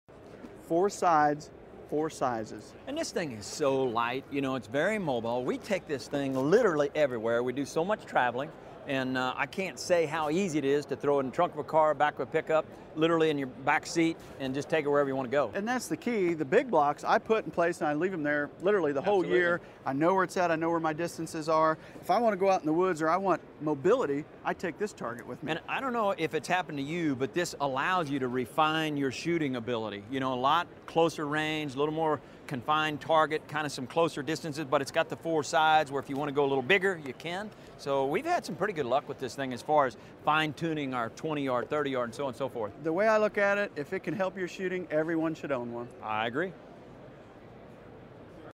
Mark and Terry Drury talk about the new Block Black
Hear what Mark and Terry Drury from Drury Outdoors have to say about the new Block Black.